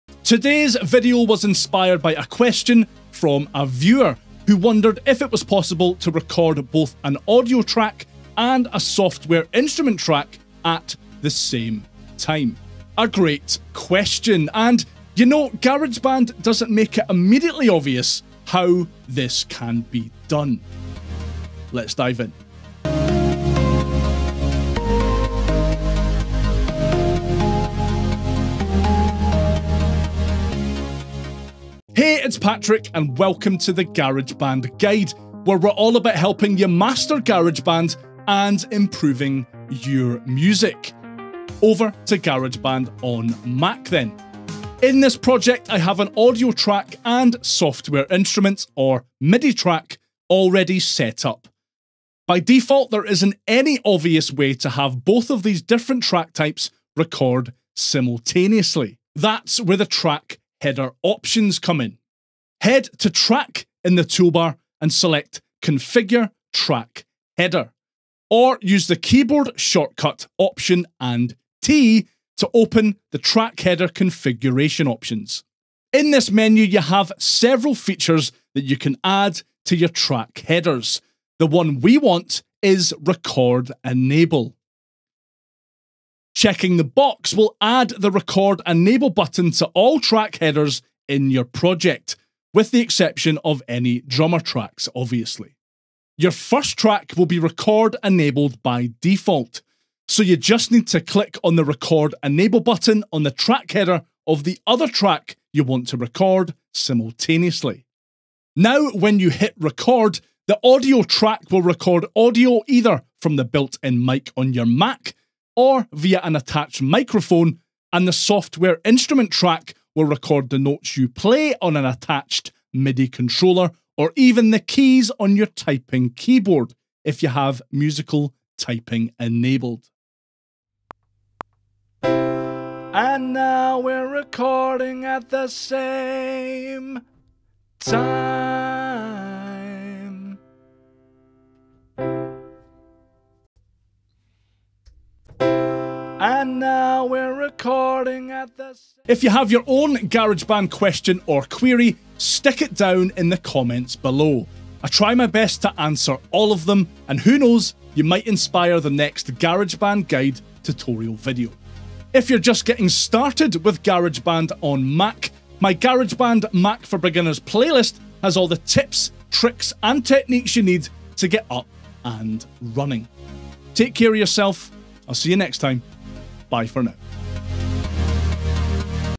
How To Record Audio and MIDI at the Same Time in GarageBand
This video was inspired by a question I received from a viewer, who wondered if it was possible to record both an Audio and Software instrument track at the same time.

A great question and GarageBand on Mac doesn’t make it immediately obvious how this can be done.

———FREE GUIDES———

———GEAR———

———SOCIALS———